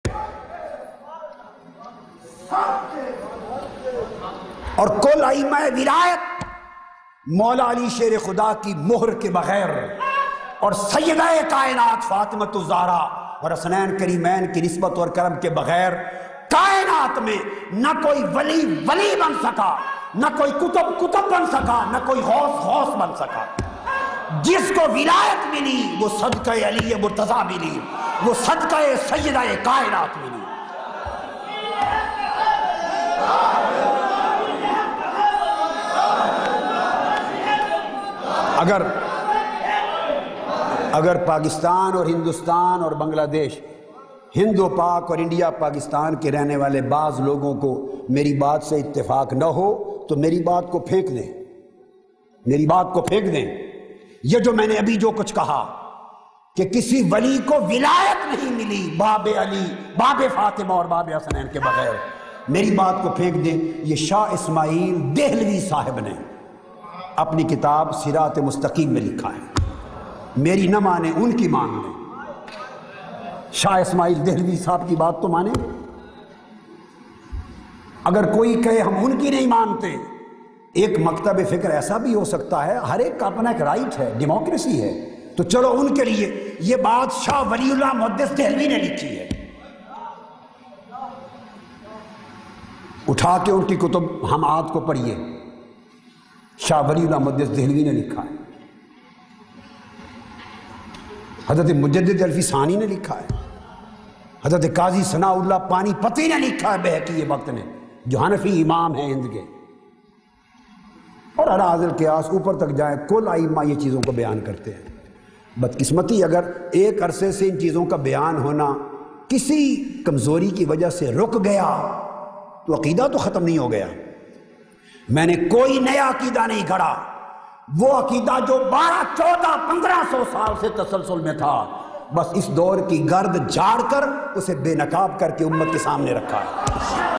(short Clip) Mohr e Ali or Wilayat
Shaykh ul Islam Dr. Muhammad Tahir ul Qadri giving lecture on
Hub e Ali (R.A) ba-Zuban e Sahaba (R.A)